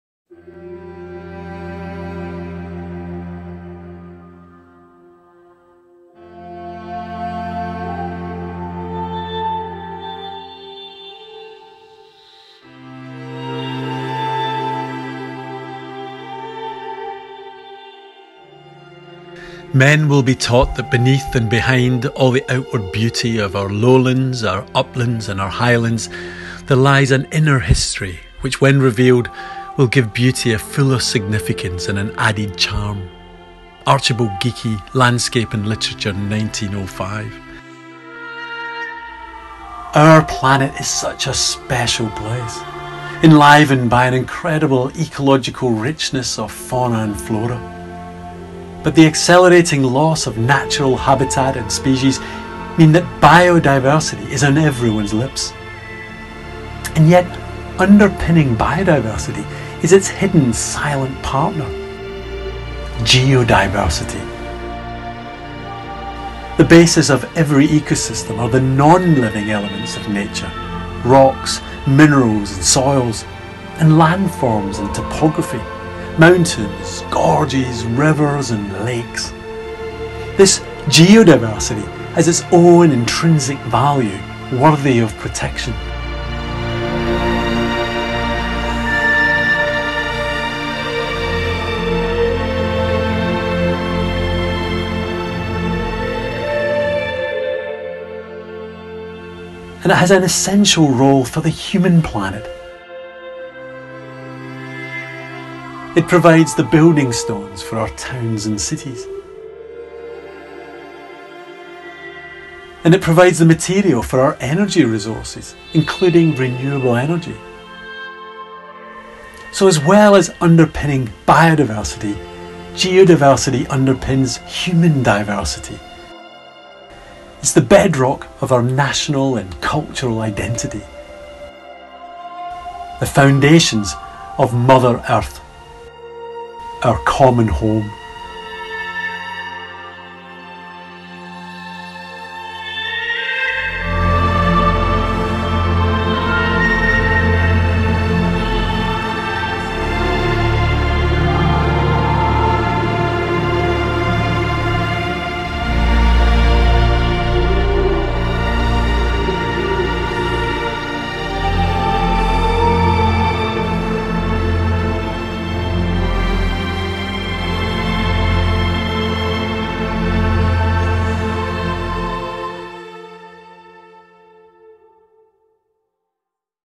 Geodiversity: Biodiversity's Silent Partner
To help support the initiative to establish an International Geodiversity Day, Prof Iain Stewart has narrated this special video. You can find out more regarding International Geodiversity Day at geodiversityday.org

We are indebted to Dr Franziska Schrodt, Dr Richard Field, Prof Iain Stewart, Dr Ozlem Adiyaman Lopes, David Baillie, Wildcat Films, the UNESCO International Geoscience Programme, the University of Nottingham, and the University of Plymouth for creating this video.